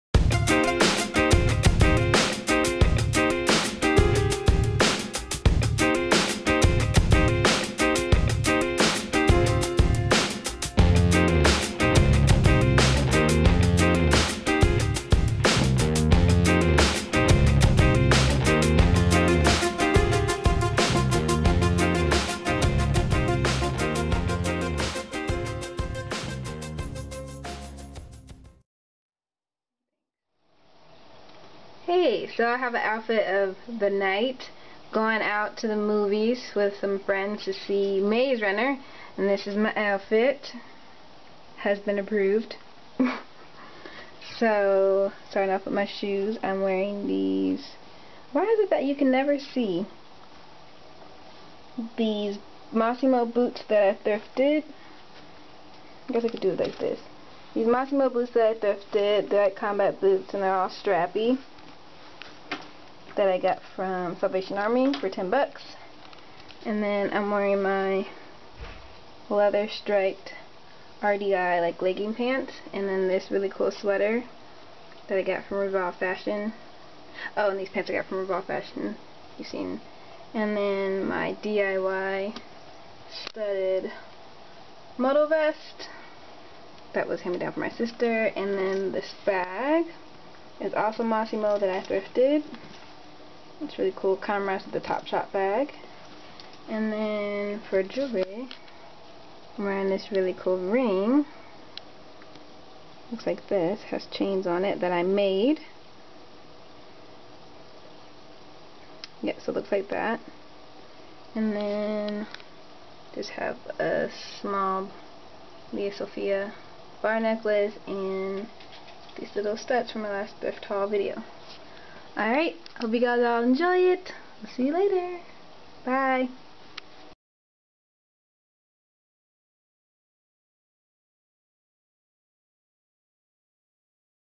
OOTN goin to the movies
Hello everyone I have a really quick OOTN from last Sunday when I went to see the movie maze runner. Which turned out to be a great film kept me guessing the whole time I was at the edge of my seat.I did not read the books but really enjoyed it. So here is what I wore. I am so sorry about the quality and the messy background , but its real life guys so yea. love you all and thanks for watching subscribing and liking my videos and sharing them.